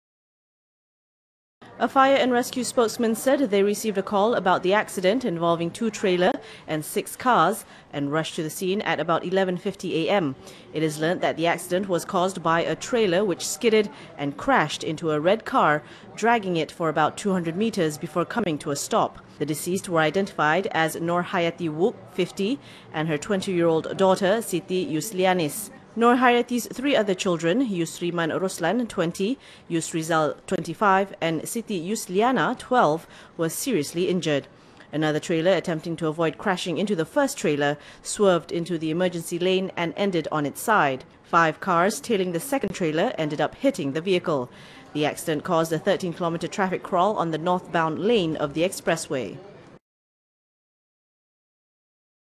Plus North-South Highway Accident 2 KILLED, 13 HURT
Two people were killed while 13 others were injured, one seriously, in an eight-vehicle pile-up along Kilometer 256.1 of the North-South Expressway, after the Menora

Tunnel near Ipoh, Perak this morning.The accident occurred as city folk headed home to celebrate Aidil Adha tomorrow.

A Fire and Rescue spokesman said they received a call about the accident involving two trailer and six cars and rushed to the scene at about 11.50 am.

It is learnt that the accident was caused by a trailer which skidded and crashed into a red car dragging it for about 200 metres before coming to a stop.

Two passengers of the car died on the spot while another was seriously hurt.

Their identities have yet to be ascertained.Another trailer, attempting to avoid crashing into the first trailer, swerved into the emergency lane and ended on its side.

Five cars, tailing the second trailer, ended up hitting the vehicle.12 people were reported hurt.

The accident caused a 13 kilometer traffic crawl on the north-bound lane of the expressway.